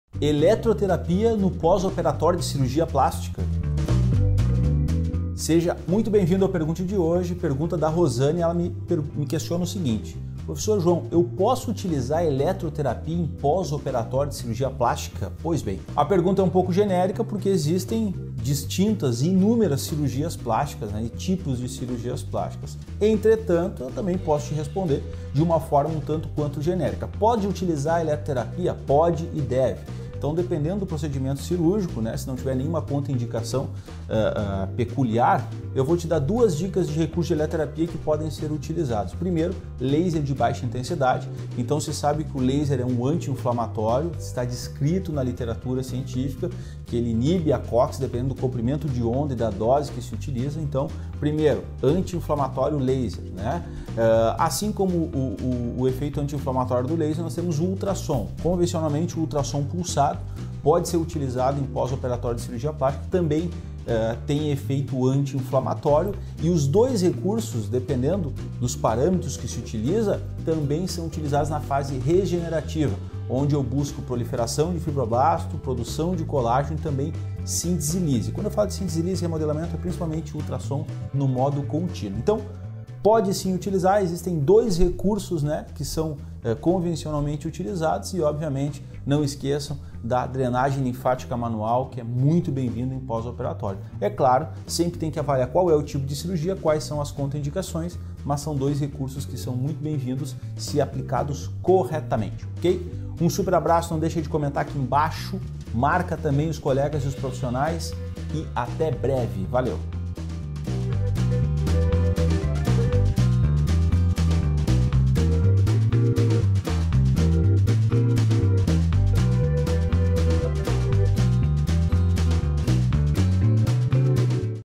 Posso utilizar eletroterapia no pós-operatório de cirurgia plástica? - Pergunte #93
Posso utilizar eletroterapia no pós-operatório de cirurgia plástica? No pergunte de hoje, eu dou duas dicas excelentes para auxiliar na inflamação pós-cirurgia.